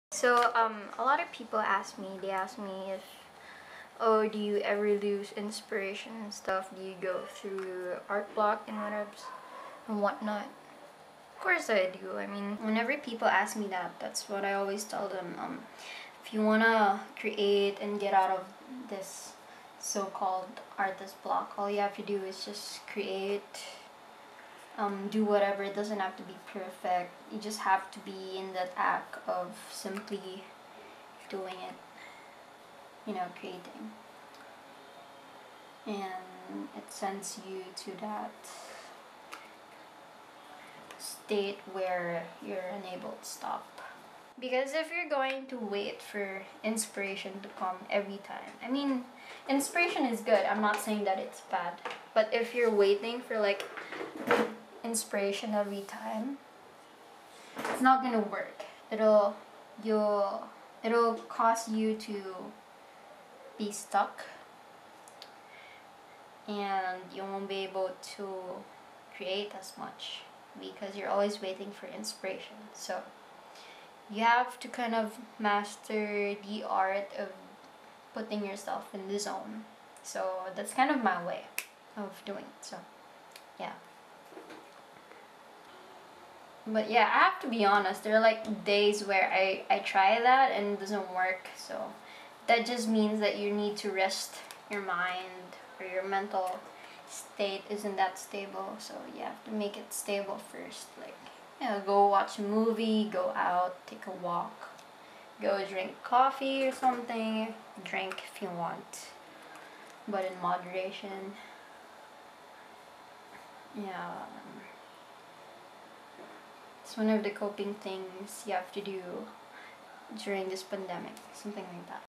2 min Advice for an Art block | Bruha Ireland
( ﾉ ﾟｰﾟ)ﾉ☀️ 2 min Advice for an Art block
Sup moots here's some advice for ya'll
(sori bout d dogs barking and me fixing stuff)